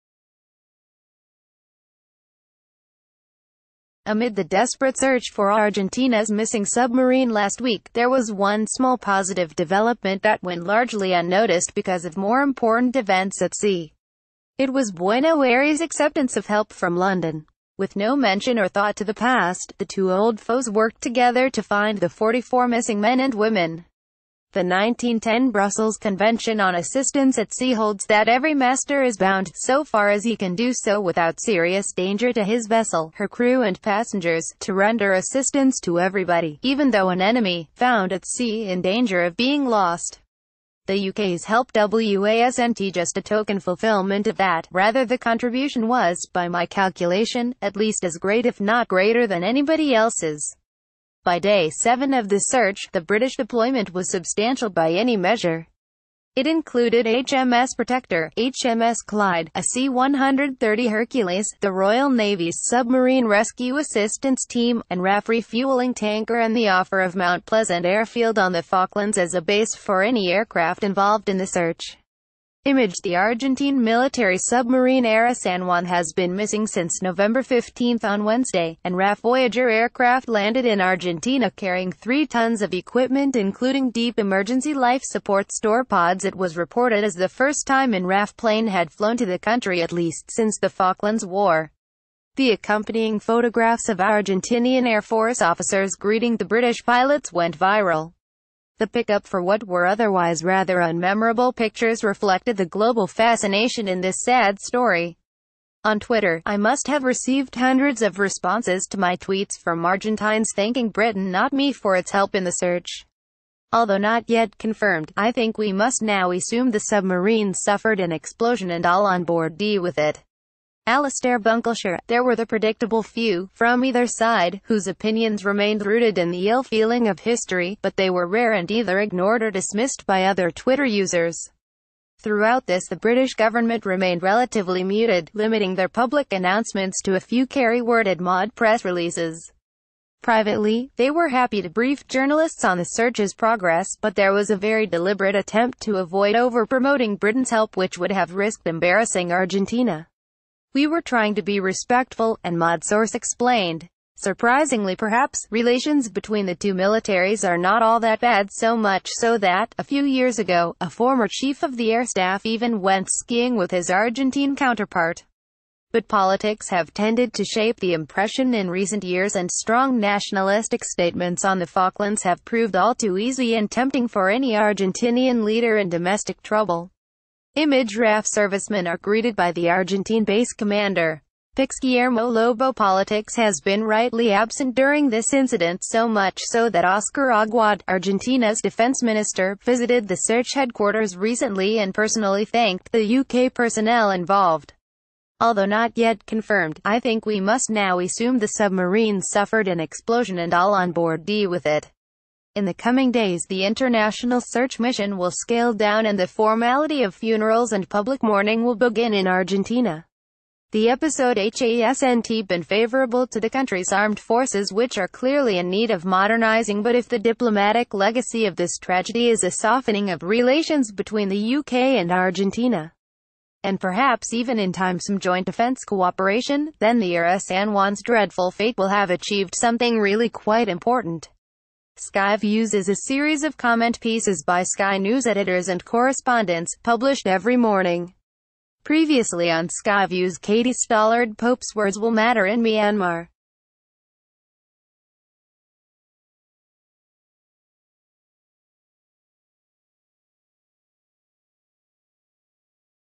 Sky views: missing sub brings old foes together| Daily News TV
Amid the desperate search for Argentinas missing submarine last week, there was one small positive development that went largely unnoticed because of ...